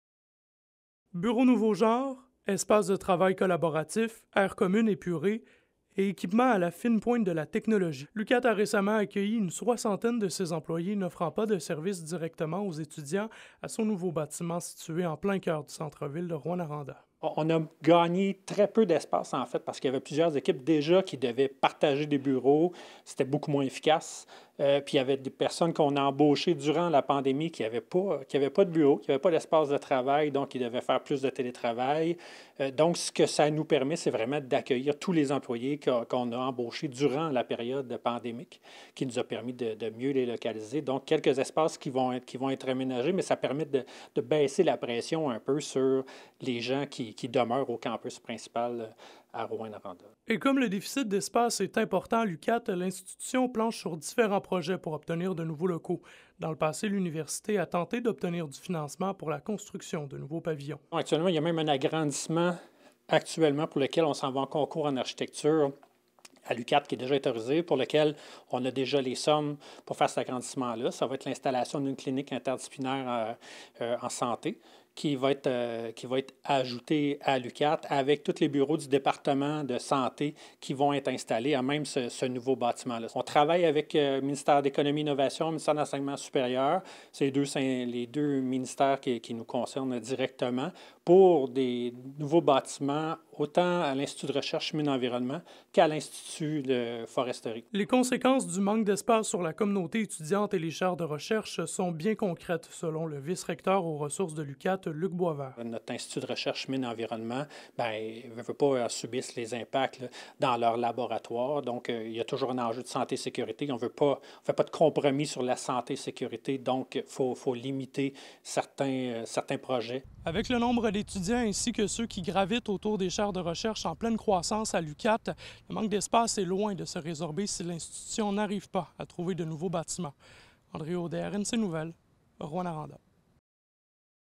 UQAT : Un nouveau bâtiment pour combler le manque d'espace
ÉDUCATION | L'Université du Québec en Abitibi-Témiscamingue est aux prises, depuis quelques années, avec un manque d'espace. L'institution fait des pieds et des mains pour tenter de pallier ce déficit.
Reportage d'Andrei Audet.